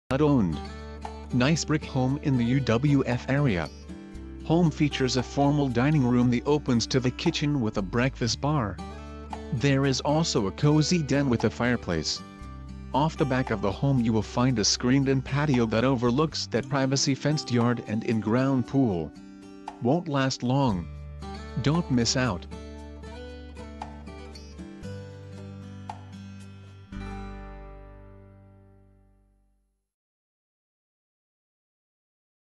1001 Stillbrook Road Pensacola FL
HUD OWNED! Nice brick home in the UWF area Home features a formal dining room the opens to the kitchen with a breakfast bar There is also a cozy den with a fireplace Off the back of the home you will find a screened in patio that overlooks that privacy fenced yard and in ground pool! Won't last long! Don't miss out!

Video uploaded on Wed 20 Mar, 2013
real estate Pensacola FL